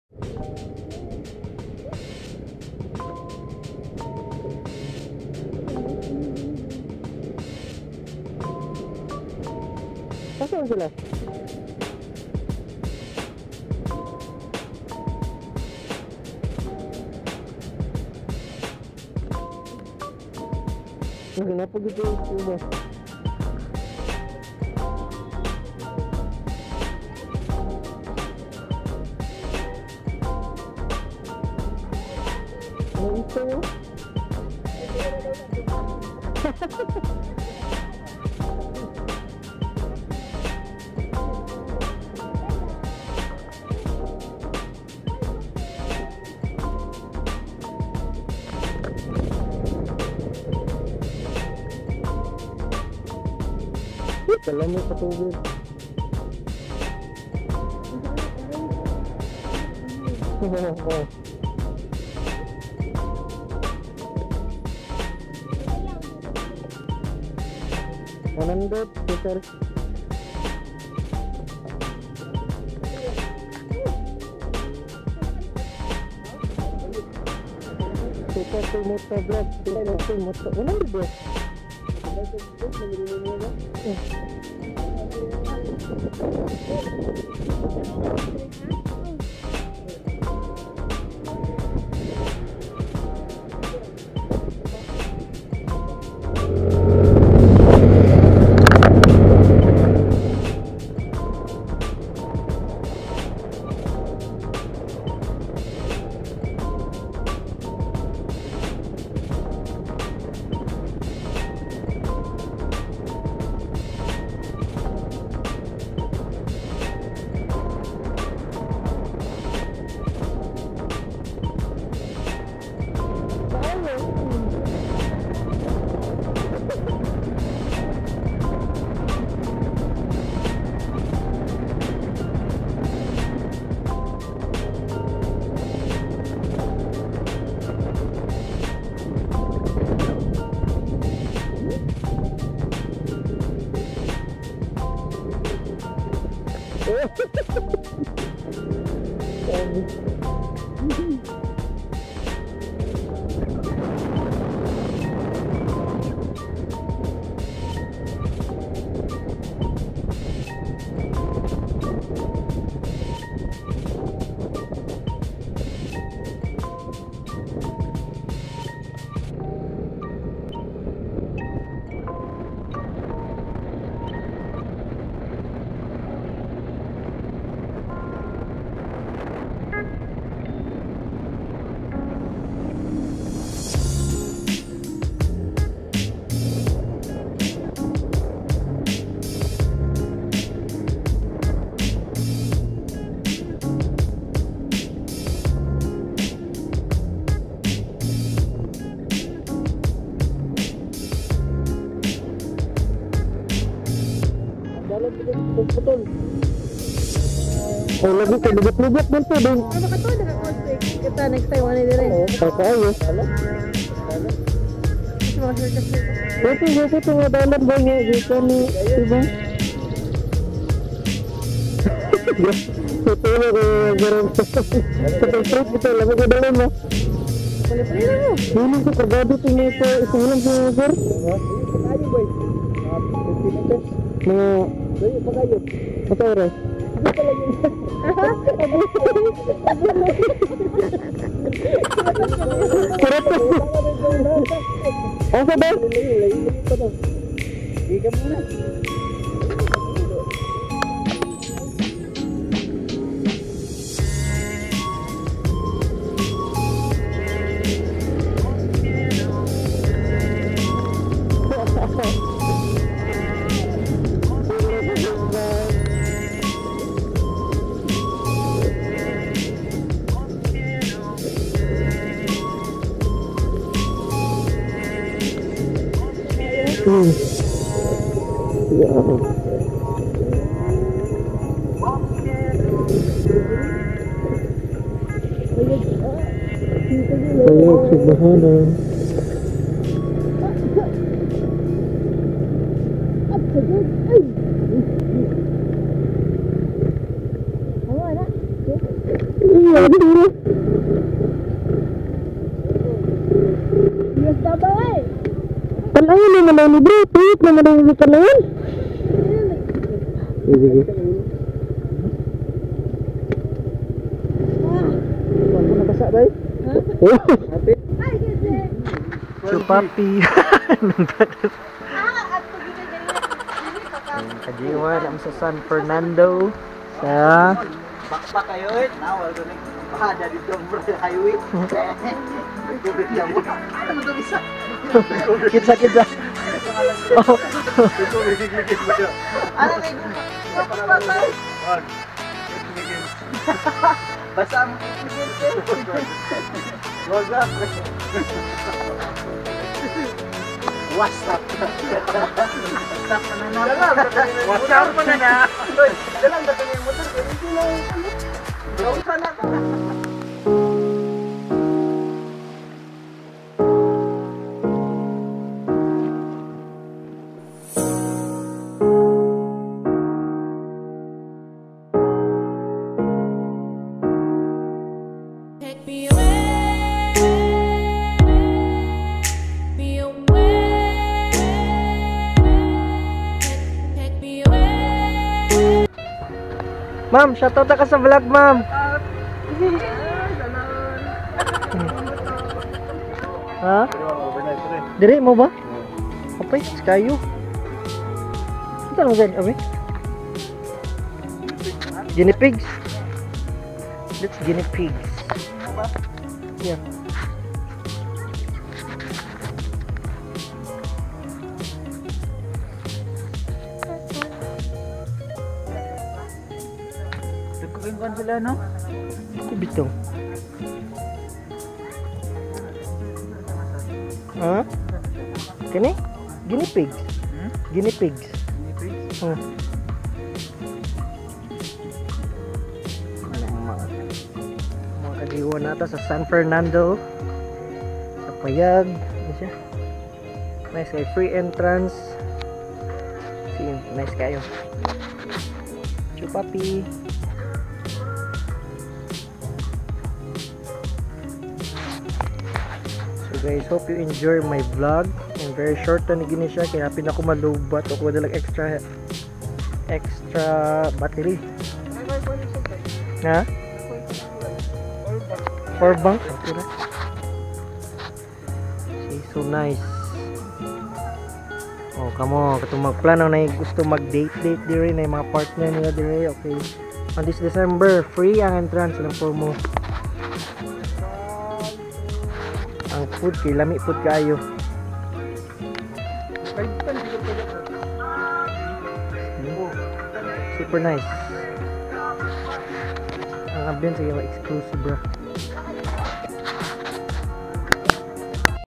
PAYAG SUGBAHANAN | SAN FERNANDO ESCAPADE | KEATZ
Team G1 Goes to San Fernando Bukidnon, visiting our friend Keats thank you bro 😊 ✌️